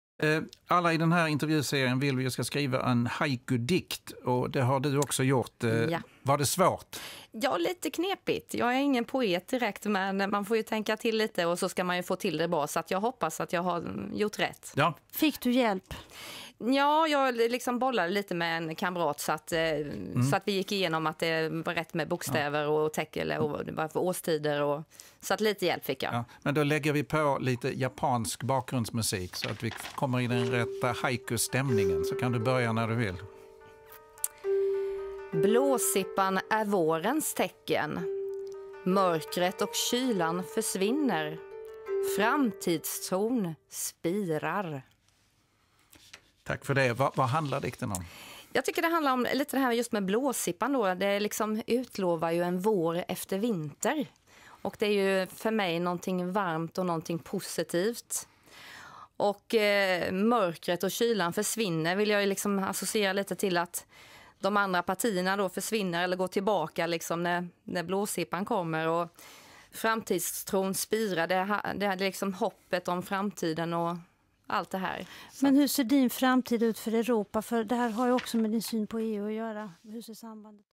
Kristina Winberg från Sverigedemokraterna
Vi fortsätter vår serie om EU-valet och idag är det Kristina Winberg från Sverigedemokraterna som kommer till studion. Varför är det så viktigt att Sverige ska lämna EU? Och varför vill hon i så fall komma in i parlamentet? Hör också Kristinas egna haikudikt och låtval.